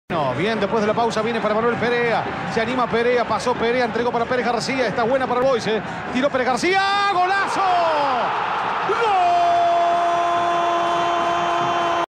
Primero de All Boys: PT 28' Pérez García.
Apertura 2010: Fecha 19

El Tomba se durmió y el Albo empató en la última bola: Godoy Cruz golpeó temprano y de la mano de una ráfaga de efectividad, ganaba 2-0 a los 7' (Rojas y Donda). Reaccionó el local y con un golazo, Pérez García descontó a los 28'. En el segundo, el Tomba desperdició muchos contragolpes, mientras All Boys buscaba el empate. Los dos equipos terminaron con 10: A los 3' complemento, expulsado Ferrari y a los 48', Sigali.

Síntesis del partido: All Boys 2-Godoy Cruz 2
All Boys: Nicolás Cambiasso; Vella, Carlos Madeo, Jonathan Ferrari y Carlos Soto; Fernando Sánchez, Hugo Barrientos y Emmanuel Perea; Pérez García; Agustín Torassa y Matos. DT: José Romero.
Godoy Cruz: Sebastián Torrico; Lucas Ceballos, Leonardo Sigali, Nicolás Sánchez y Torres; Roberto Russo, Mariano Donda, Israel Damonte, y Ariel Rojas; Jesús Vera y César Carranza. DT: Omar Asad.
Goles: PT 56" A. Rojas; 7' M. Donda; 28' Pérez García. ST 48' M. Matos (penal).
Expulsados: ST 3' J. Ferrari; 48' L. Sigali.
Cambios: ST 17' V. López por Perea; 22' S. Sánchez por Donda; 26' Miranda por Ceballos; 29' Ereros por Pérez García; 34' Dutari por Vera; 36' Zárate por Vella.
Arbitro: Federico Beligoy.